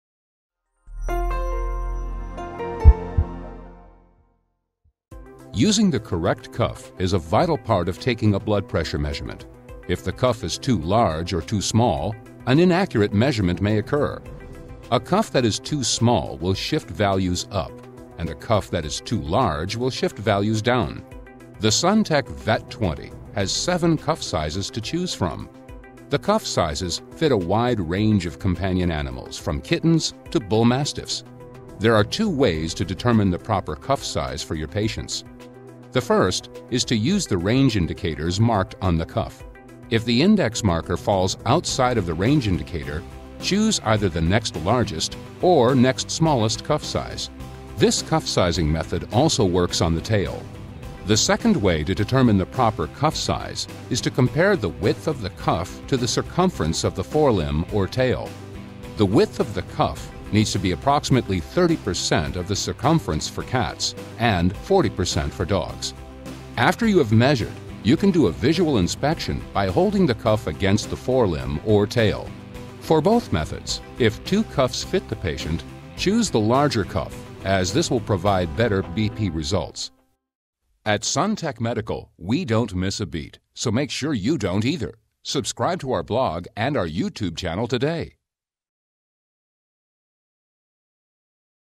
SunTech Vet BP: Selecting the Correct BP Cuff Size (1 of 4)
Instructional video to assist veterinary staff on choosing the correct BP cuff size for placement on the front forelimb or the animal’s tail when taking BP measurements on cats and dogs.